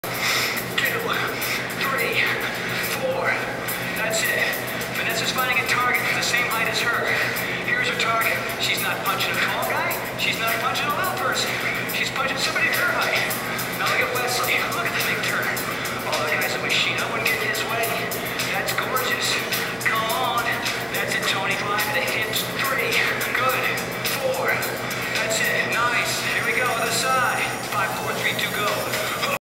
Hook and Uppercut
It's been a really long time since I've had any formal training in boxing or stand up martial arts so if you don't mind...keep the comments positive :) or at least realize as you're being negative that I'm aware that I'm not exactly perfect on my form.